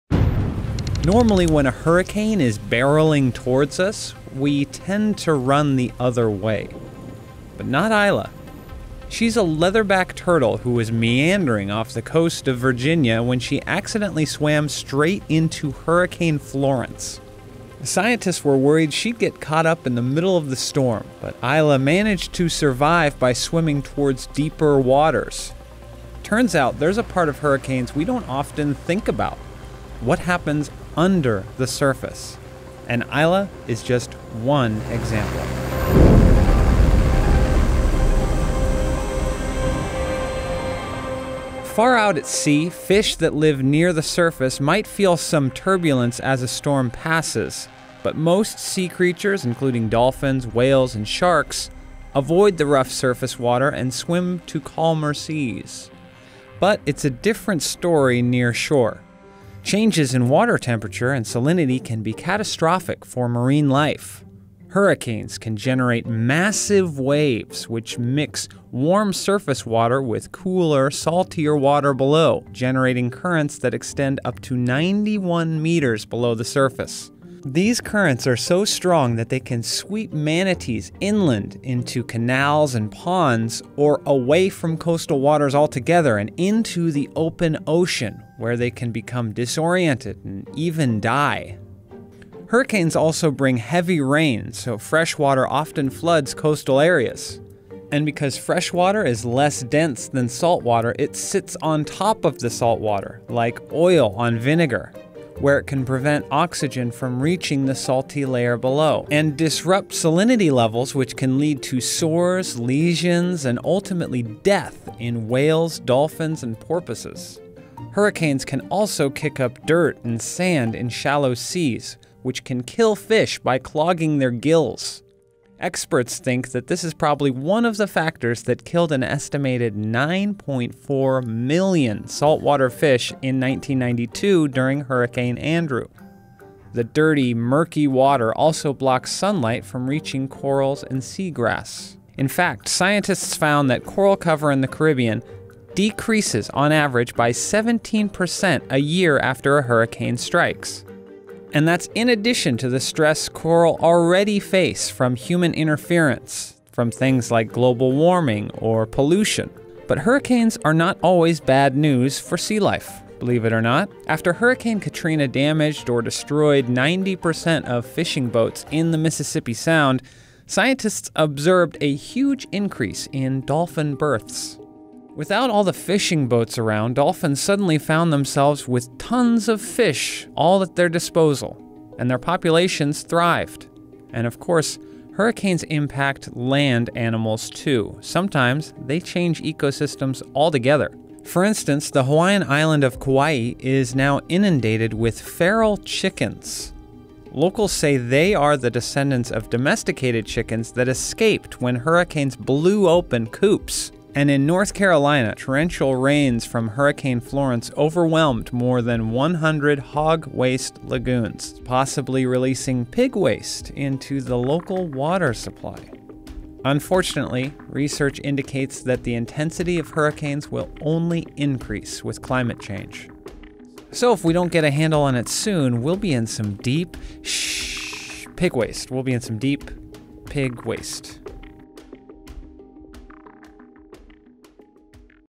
What Happens To Fish During A Hurricane
Hurricanes can be just as deadly for marine life, sometimes stranding them on land or far out at sea. But sometimes marine life benefits or even thrives after these extreme weather events.

Following is a transcript of the video: